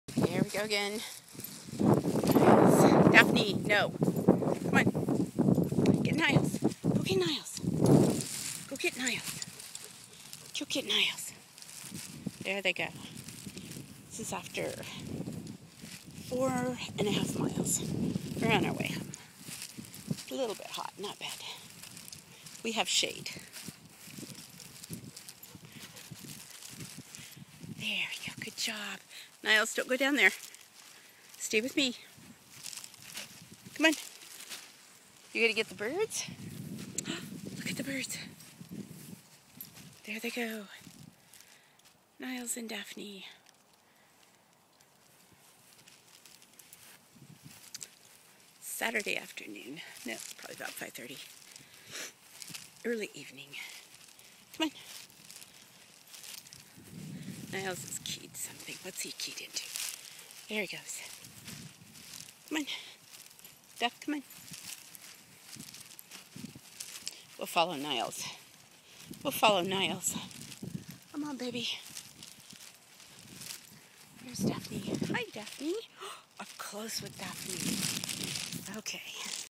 Dog walk
Niles and Daphne end of their daily  mile walk